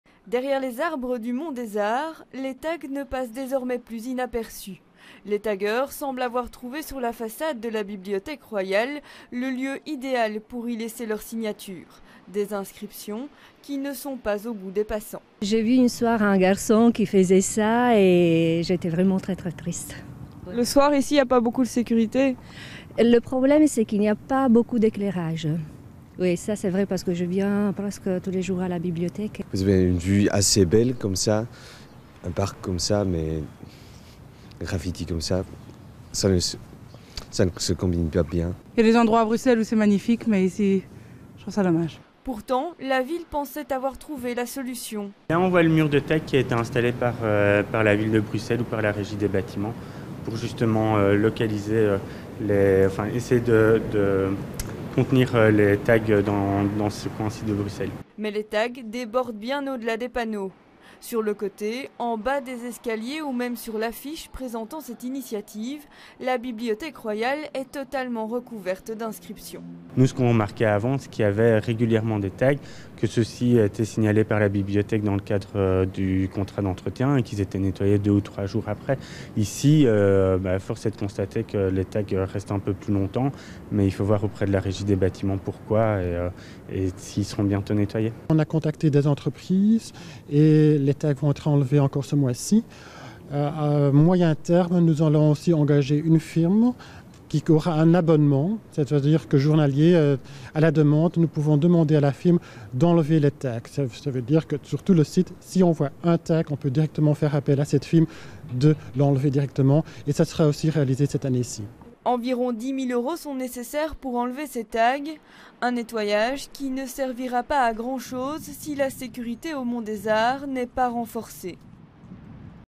Des tags sur les facades de la Bibliothèque royale Mont des Arts
Certains murs de la Bibliothèque royale au Mont des Arts sont fortement taggués.